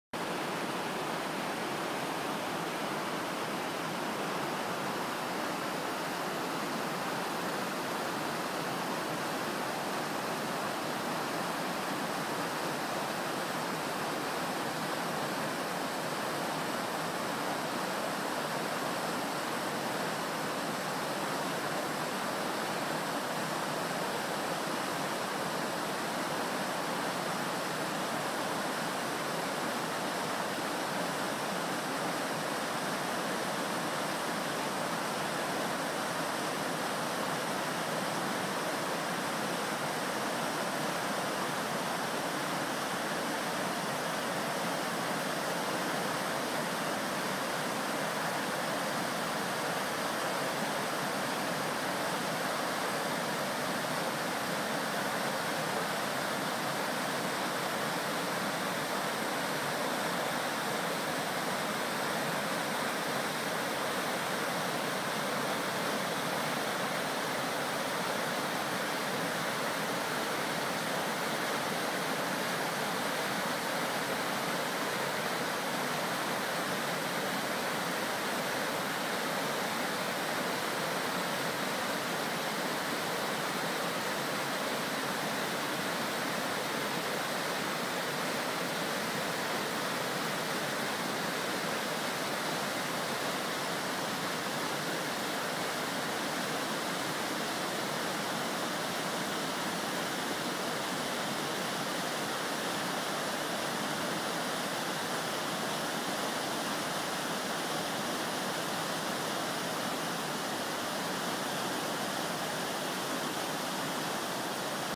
le pot bouillant
ce "saut" sur la Benaize mérite bien son nom